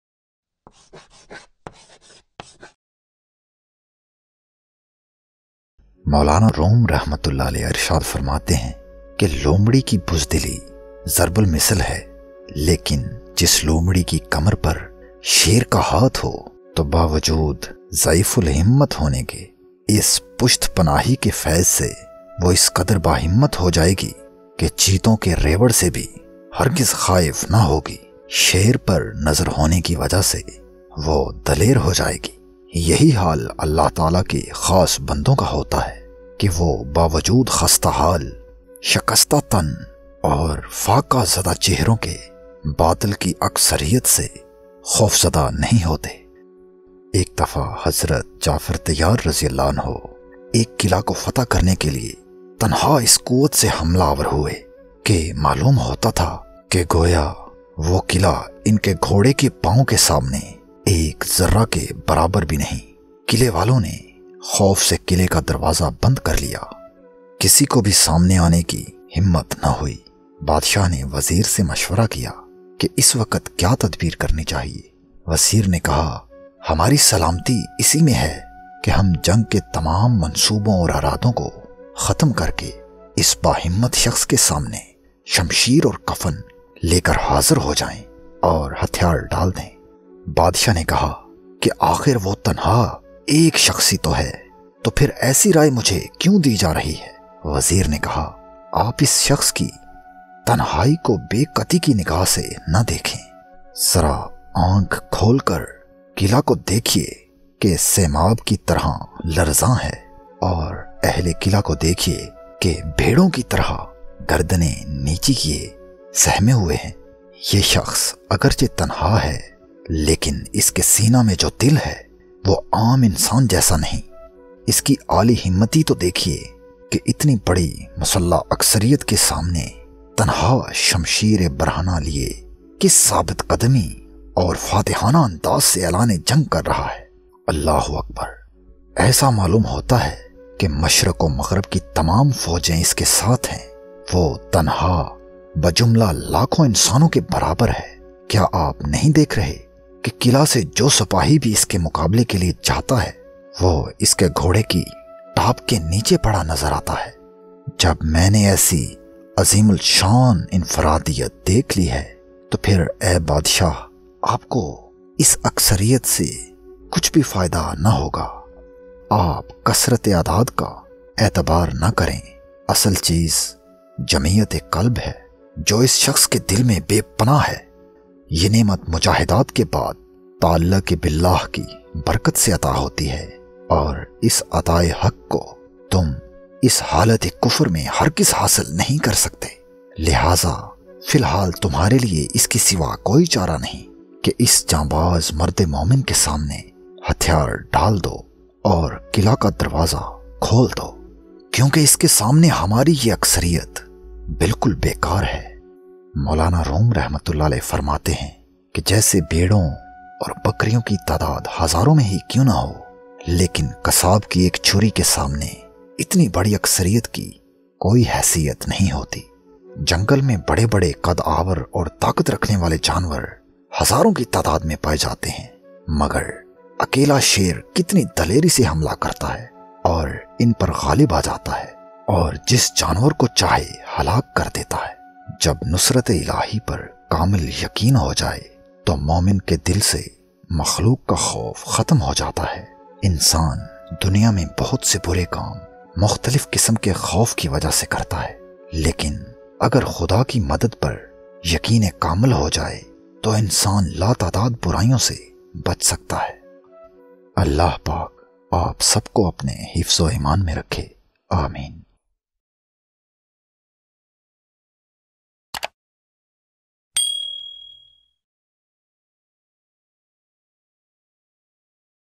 Hazrat Jafar Tayyar Story | Hakayat Rumi Urdu Hindi | Demystified Islam
This story is taken from the book ''Masnavi-e-Rumi".Hazrat Jafar Tayyar Story | Hakayat Rumi Urdu Hindi | Demystified Islam.urdu story hakayat Molana Rumi R.A.

●▬▬▬▬▬▬▬▬ஜ۩۞۩ஜ▬▬▬▬▬▬▬▬●

Follow our Facebook page for more Islam posts,Whatsapp status and Sufism videos